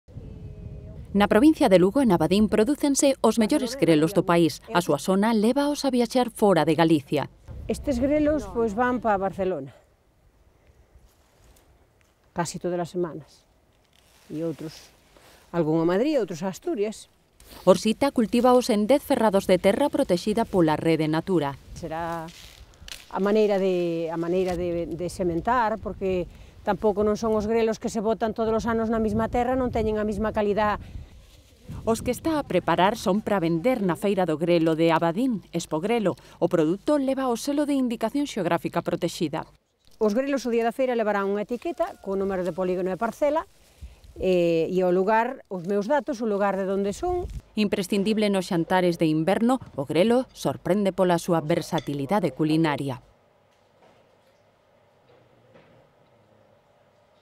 Os mellores grelos do país
Na provincia de Lugo prodúcense os mellores grelos do país.